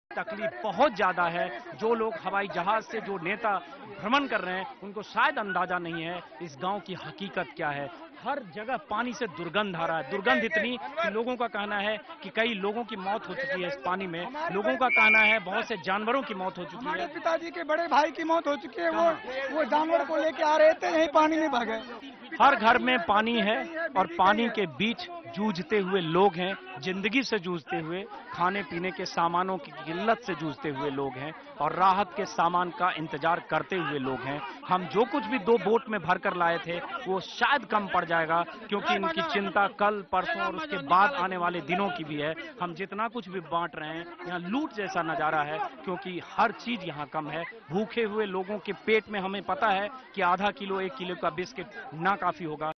Ajit Anjum reporting with social responsibility during Bihar flood ( 5)
'Rahat Lekar Chalo Bihar', this was the campaign of 'News24' to help the people of Bihar, who were badly hit by Kosi flood. Managing Editor of News24 Mr.Ajit Anjum went there with food and other necessory things and helped the victims...